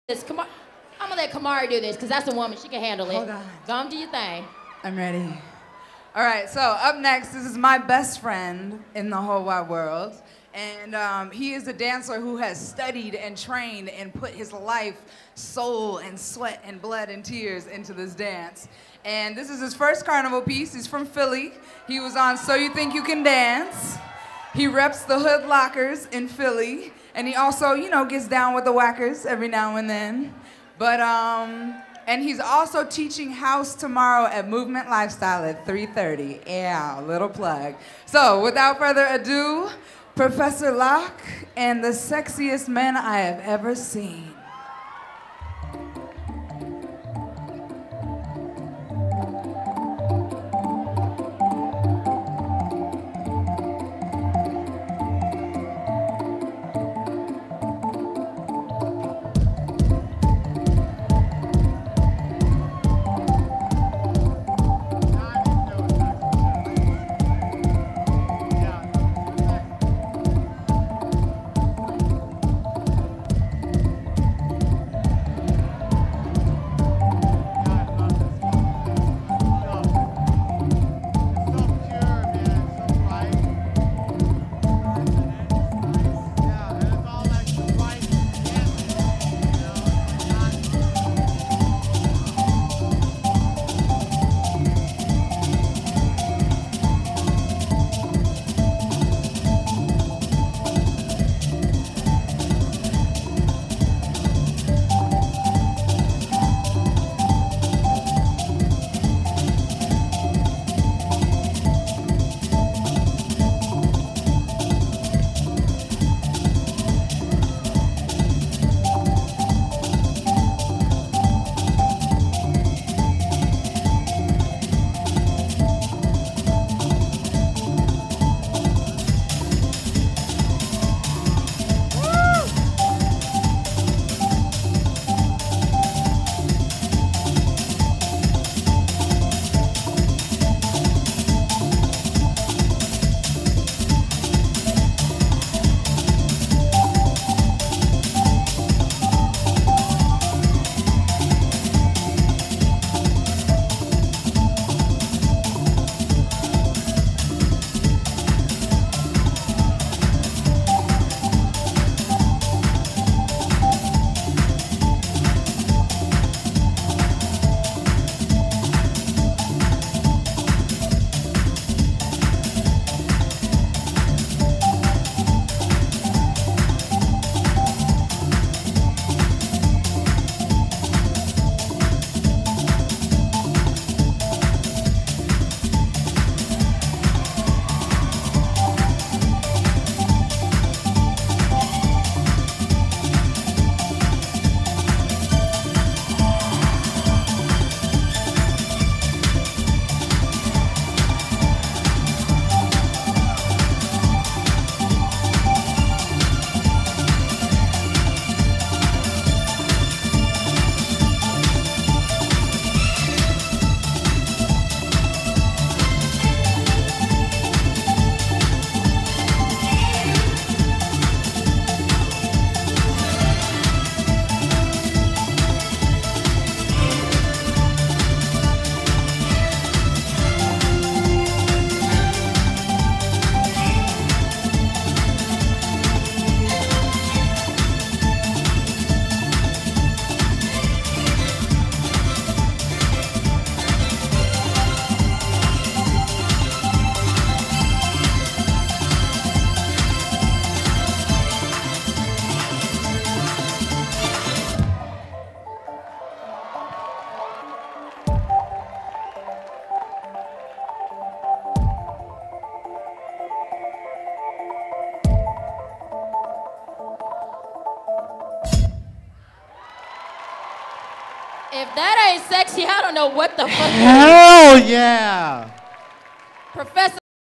PROFESSOR LOCK CARNIVAL PIECE 2013 (Feat. Tony Tzar & King Charles)
My Debut piece into the L.A. dance scene on the Carnival stage.

Choreography: myself

Dancers: Tony Tzar, Aris Jefferson,  Jamz, Eddie Martinez, Amra Ricketts, and King Charles